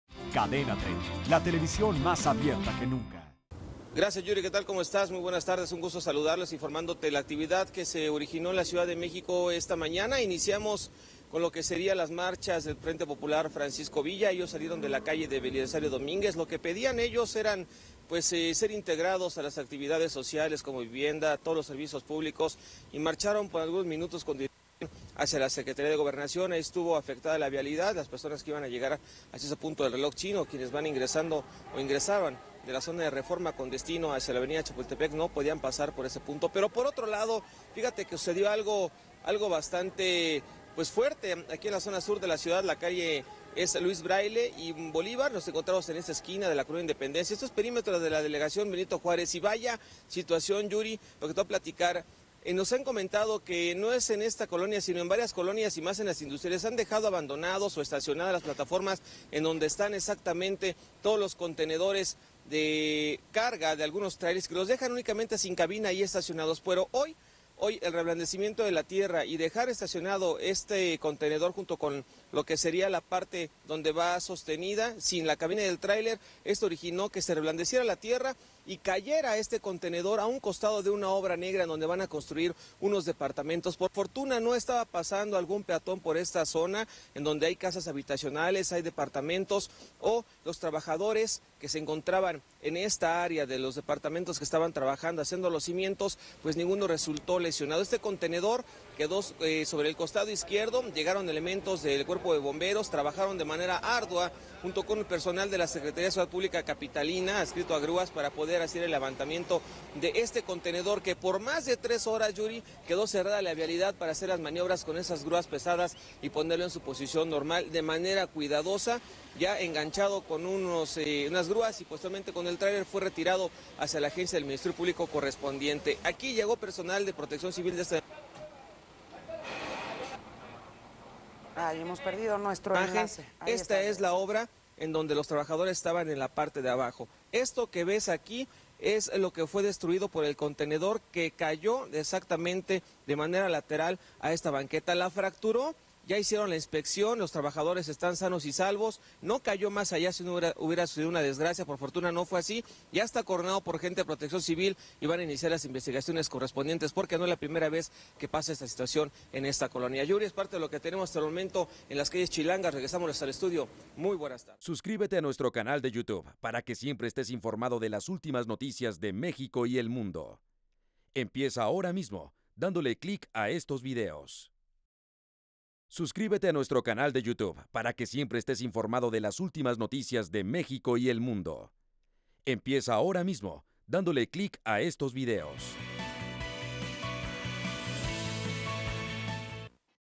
Cae contenedor de un trailer a un costado de una obra negra
En la ciudad de México comenzaron las marchas del frente Popular Francisco Villa para pedir ser integrados a los servicios públicos. En la Benito Juarez han dejado contenedores de trailer abandonados y uno cayo en una obra negra.